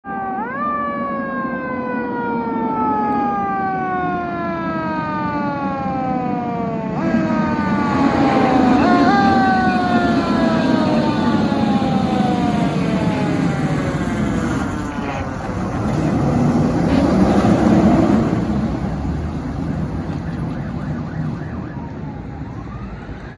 MTFD Ladder 160 31-6-91 & 1st Assistant Chief Car 102 31-1-68 Responding 1/10/21
Location: Middletown NJ
Call Type: Fire Alarm
Unit in Video: Ladder 31-6-91 & Chief 31-1-68
Date Filmed: 1/10/21
Additional Info: N/A